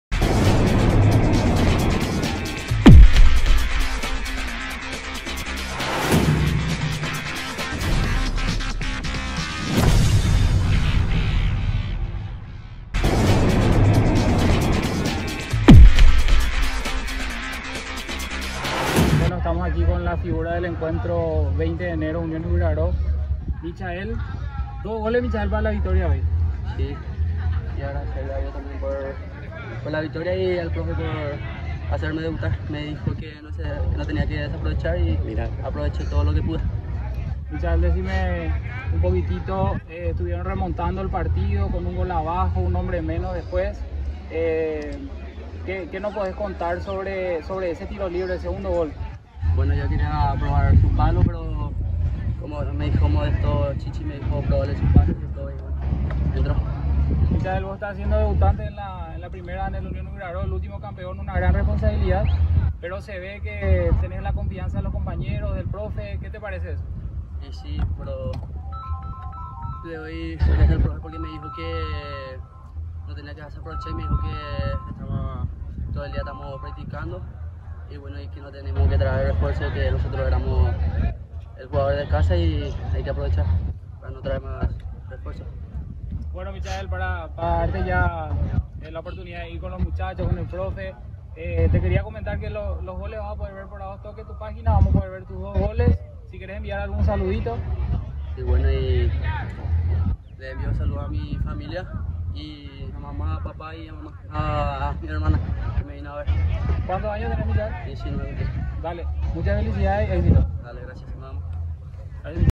MICHAEL SANCHEZ FIGURA DEL PARTIDO 20 DE ENERO VS UNION YBYRARO - LCF 2022
Entrevista a la figura del Partido Michael Sanchez del Club Unión Ybyraro, en el Partido por la Fecha 1 de la Liga Capiateña de Futbol
Dónde se llevaron la victoria por el resultado de 2 - 1